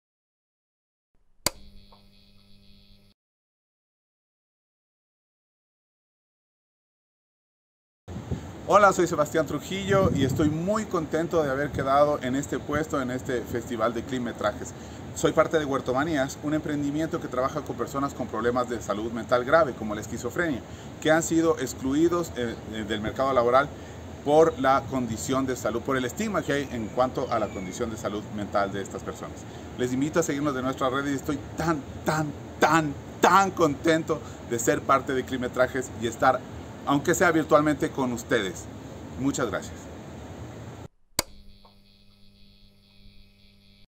Sebastián Trujillo. 3º Premio Festival de Clipmetrajes XIII ed.
En este vídeo, Sebastián Trujillo, ganador del premio al Tercer Mejor Clipmetraje, nos manda un mensaje desde Quito agradeciendo el formar parte del palmarés ganador.